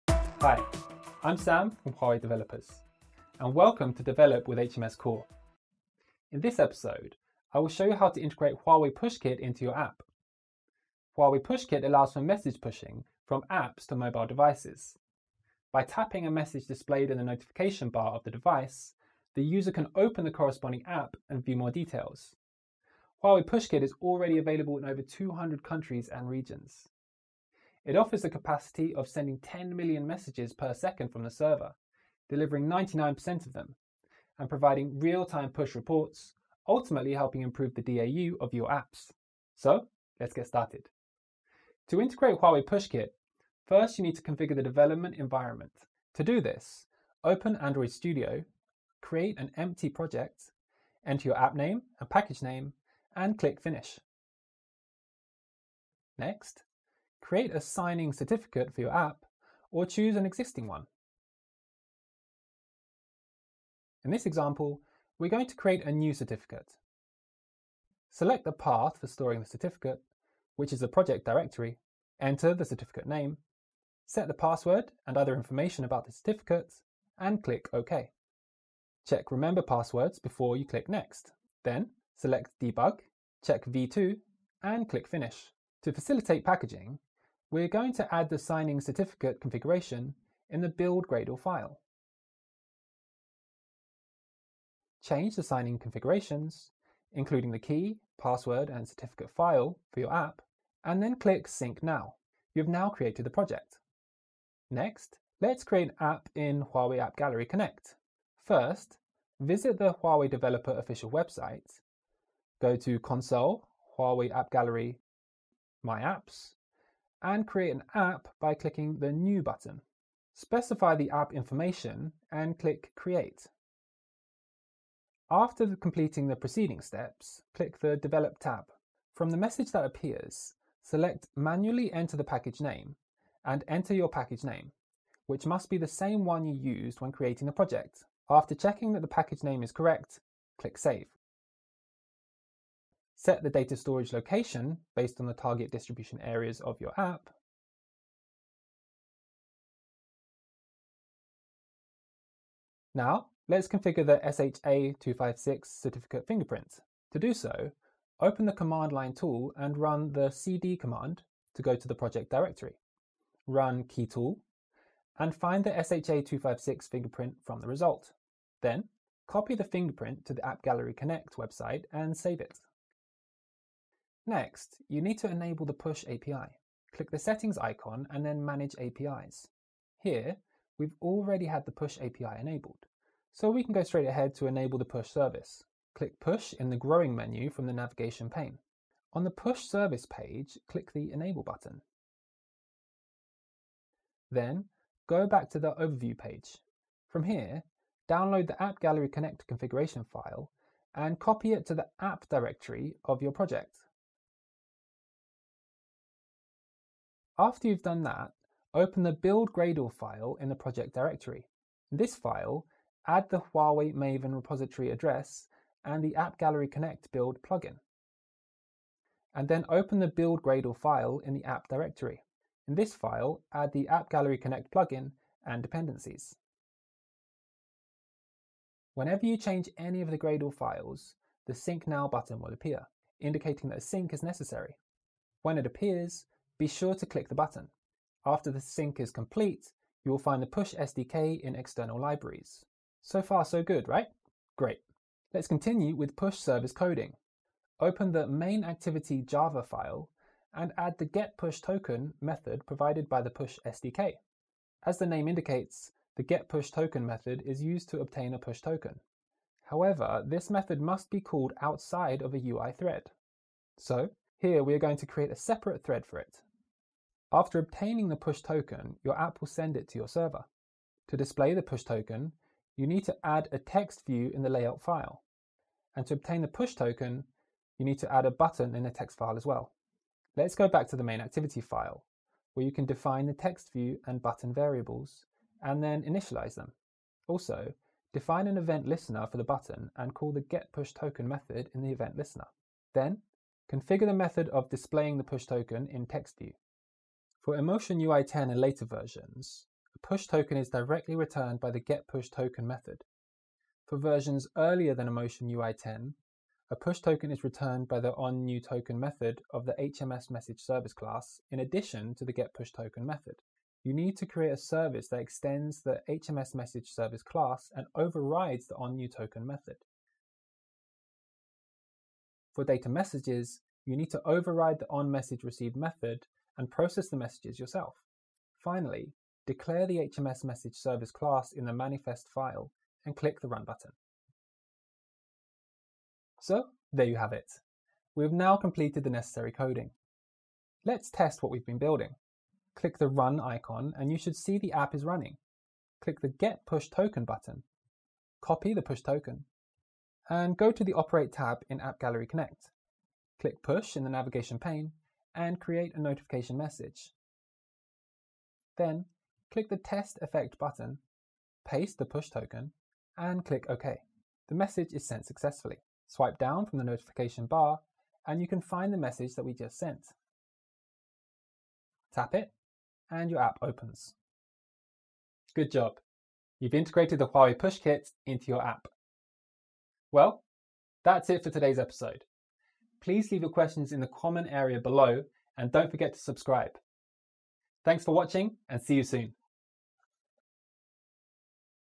HMSTips: How to Integrate with HMS Core - HUAWEI Push Kit
This video introduces the knowledge and quick integration of HUAWEI Push kit. In this  integration guide, we introduce the code details of push text to help developers quickly integrate HUAWEI Push Kit.